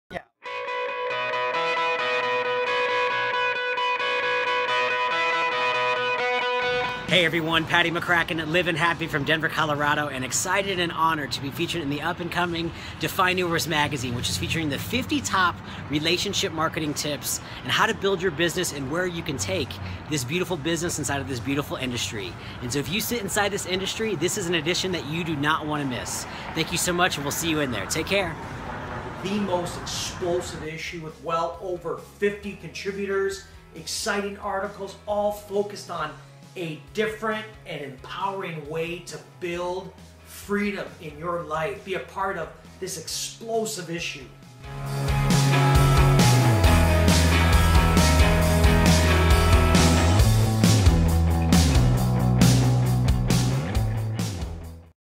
Defyeneurs Paddy McCracken
Super excited about publishing Paddy McCracken in the Top 50 Relationship Marketing Tips - the most explosive issue of Defyeneurs.